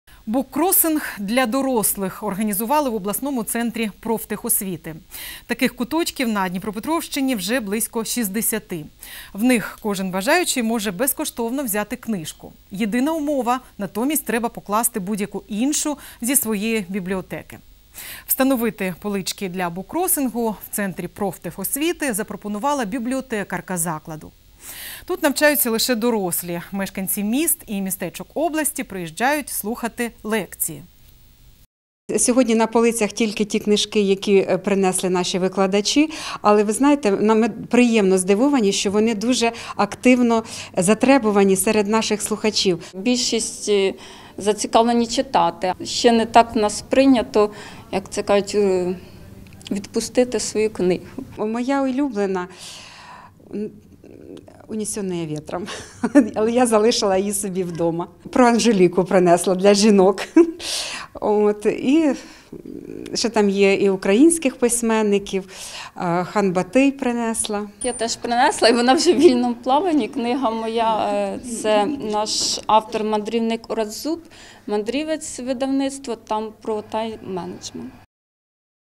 Буккросинг для дорослих з'явився в обласному центрі профтехосвіти
Буккросинг для дорослих відкрили в обласному центрі профтехосвіти. Це вже майже 60-й куточок на Дніпропетровщині, в якому кожен бажаючий може безкоштовно взяти книжку. Єдина умова: натомість треба покласти будь-яку іншу зі своєї бібліотеки. Встановити полички для буккросингу в центрі профтехосвіти запропонувала бібліотекарка закладу. Тут навчаються лише дорослі: мешканці міст і містечок області приїжджають слухати лекції.